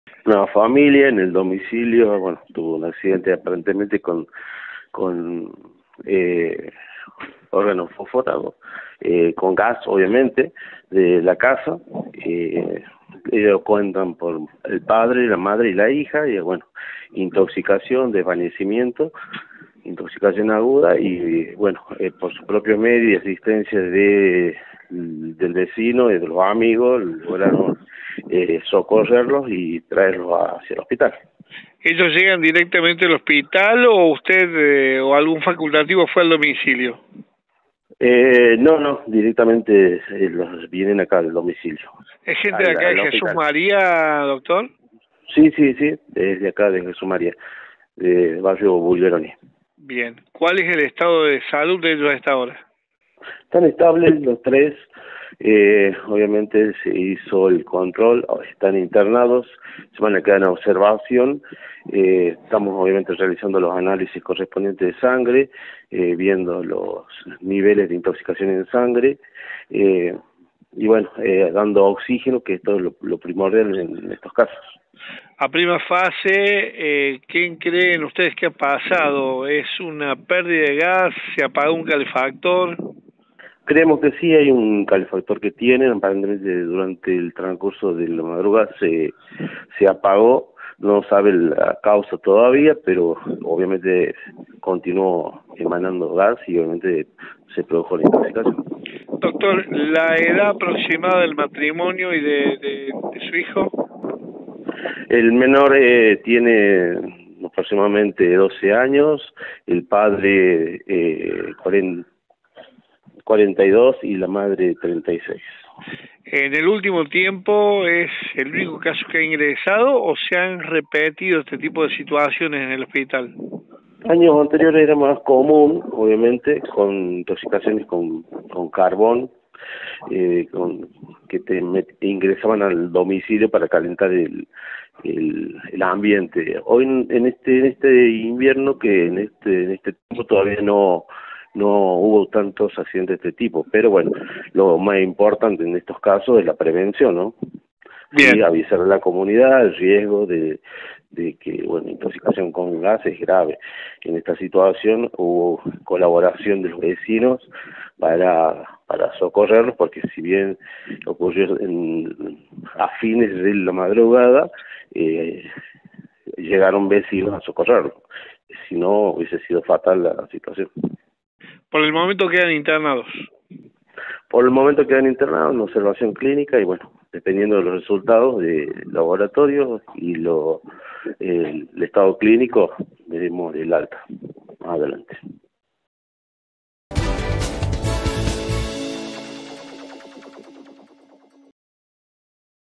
Una familia de Jesús María se intoxicó con monóxido de carbono
🏥 Una familia de barrio Bulgueroni de Jesús María fue hospitalizada por intoxicación con monóxido de carbono.

La situación fue advertida por los vecinos que los socorrieron y los llevaron al nosocomio local.

El matrimonio y su hijo de 12 años, quedarán en observación hasta que se conozcan los resultados de los análisis respectivos. Aparentemente, se habría apagado un calefactor en la vivienda y eso habría generado el escape de gas.

🤳 Dr. José Pérez, vice director Hospital Vte. Agüero.